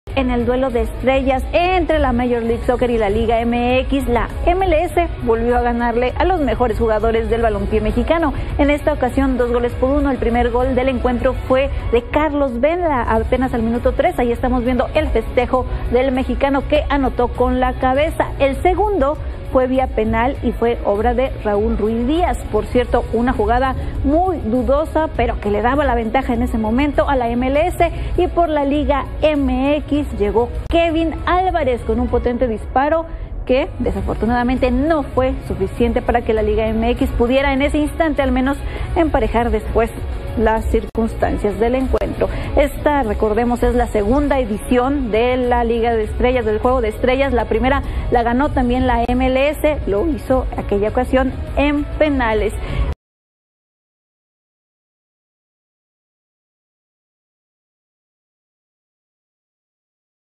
MLS vence a la Liga MX en el Juego de Estrellas por segundo año consecutivo
El conjunto estadunidense tuvo mejor eficacia y hasta la ayuda del VAR; mientras a los mexicanos les falló mucho la contundencia